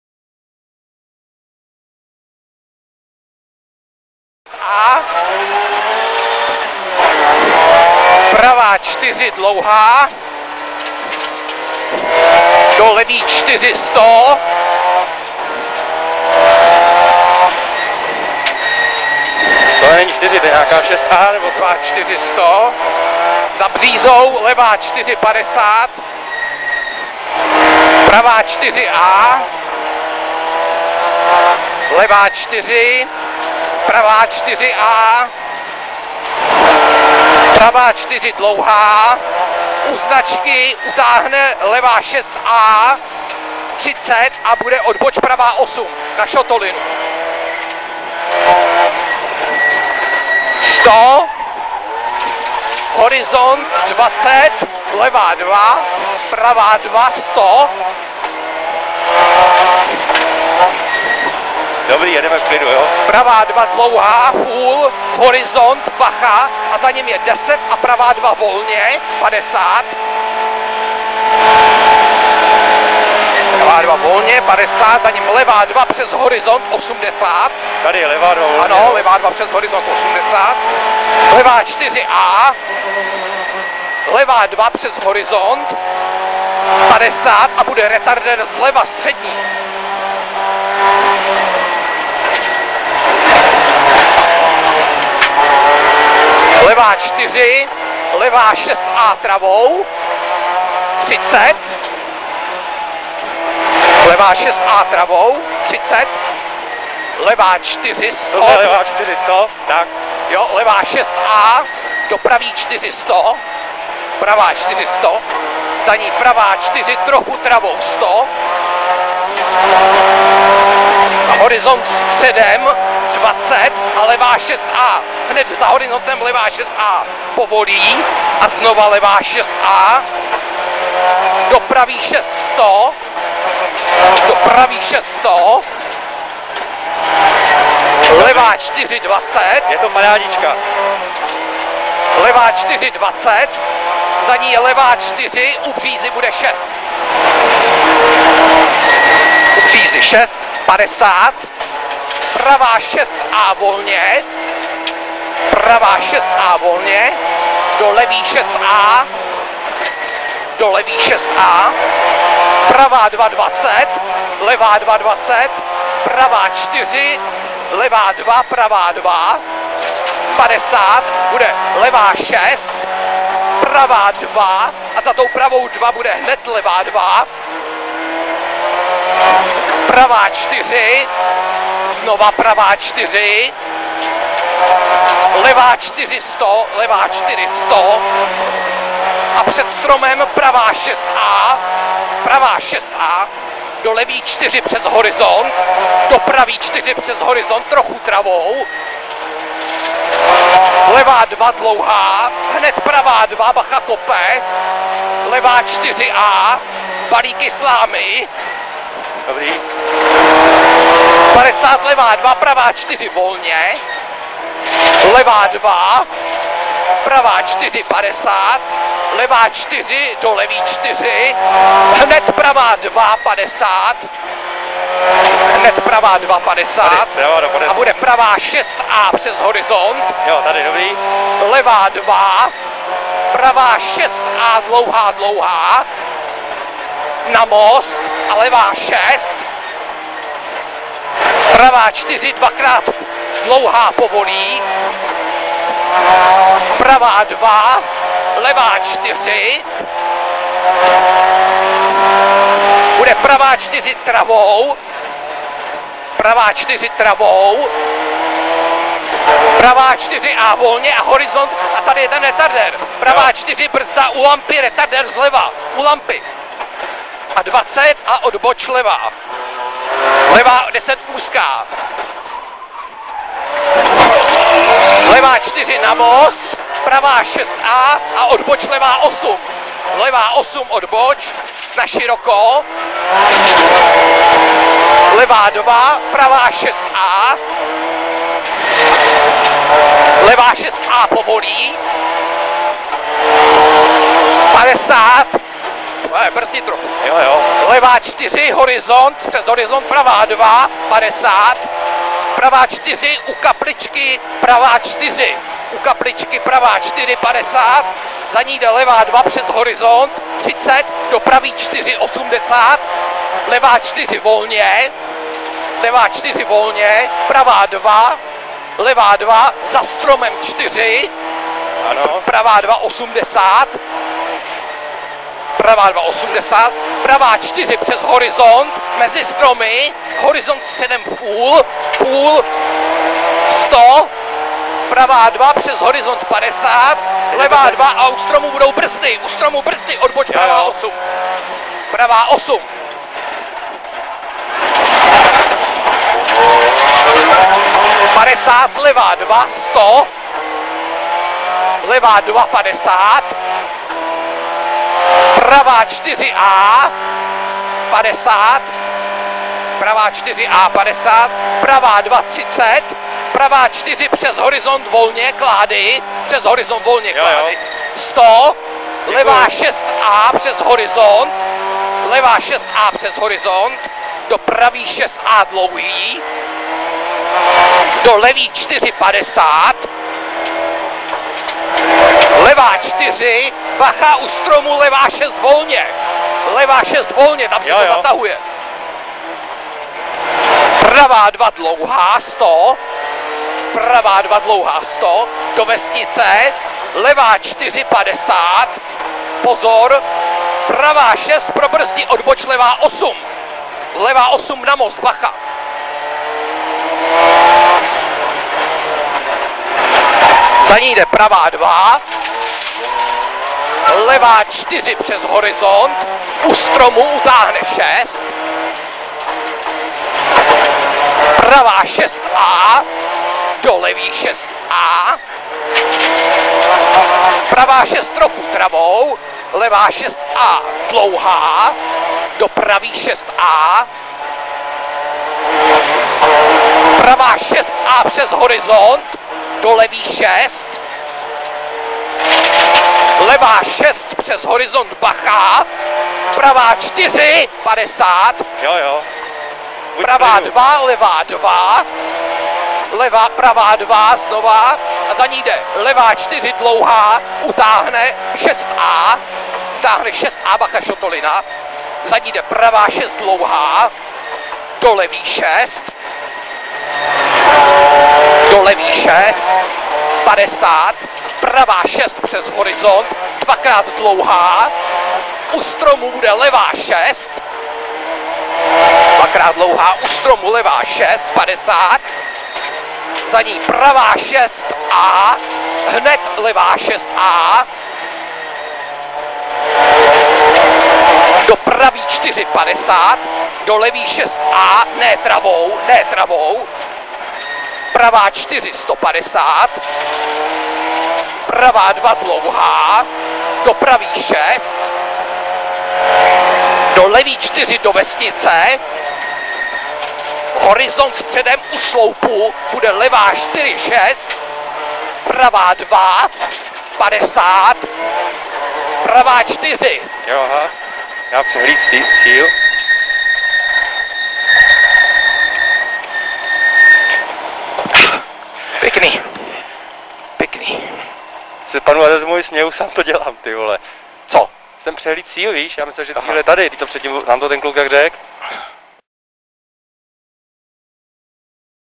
JIří Navrátil - On-board video RZ1 Vysočina 2010
Onboard video posádky Jiří Navrátil - Jiří Káňa ve voze Ford Sierra RS Cosworth v Rally Vysočina 2010.